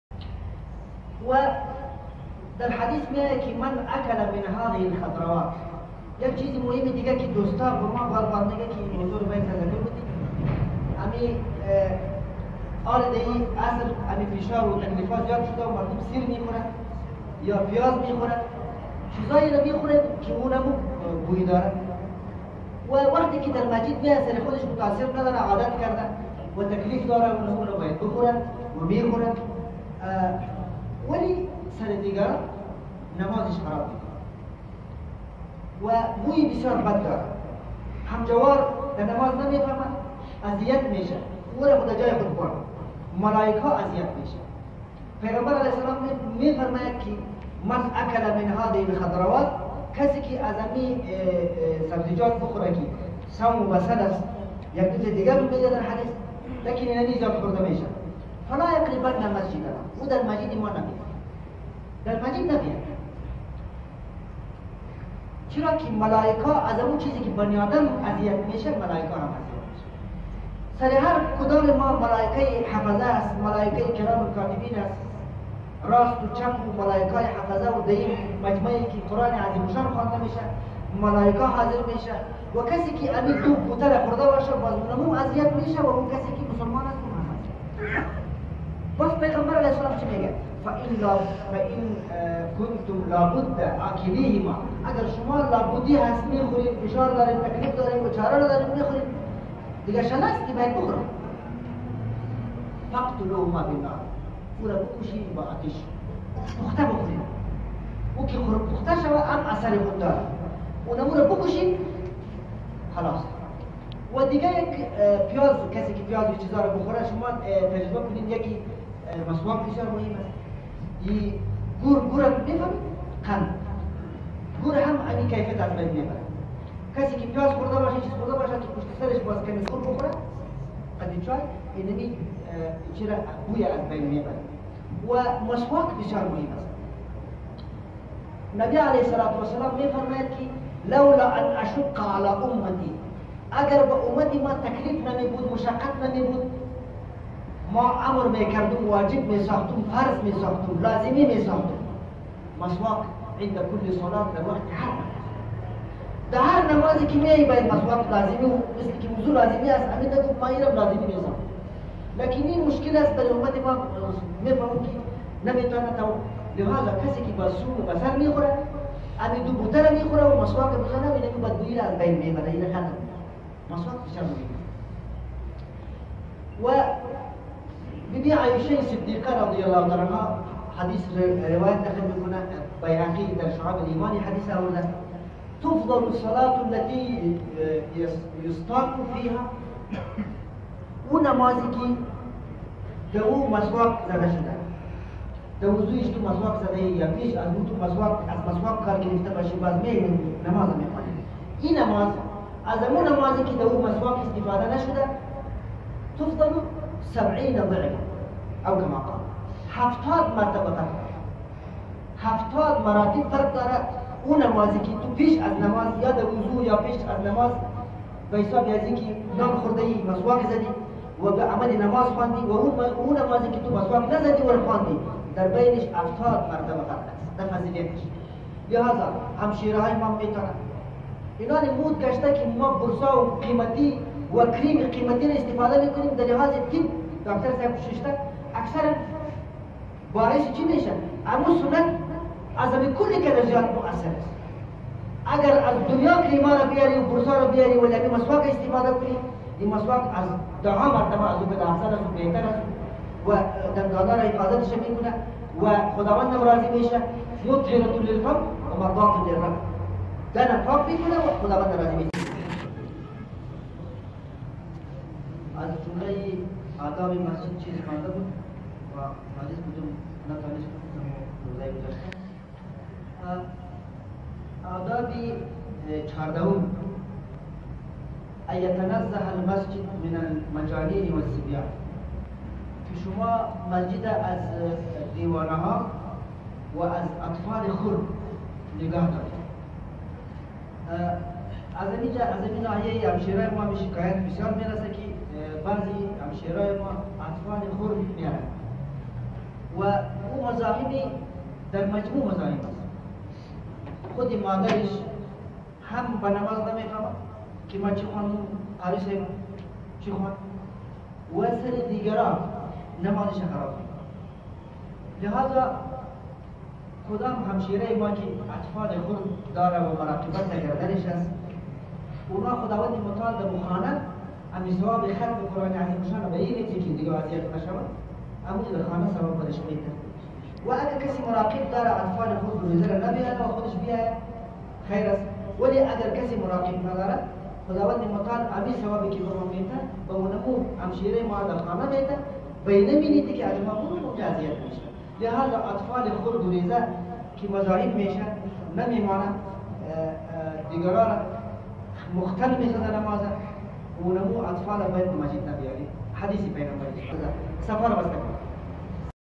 خوردن پیاز و سیر قبل از رفتن به مسجد و اذیت دیگران | تراویح 2019
خوردن پیاز و سیر قبل از رفتن به مسجد و اذیت دیگران
جریان تراویح در یکی از مسجد
تراویح در یکی از مساجد افغانستان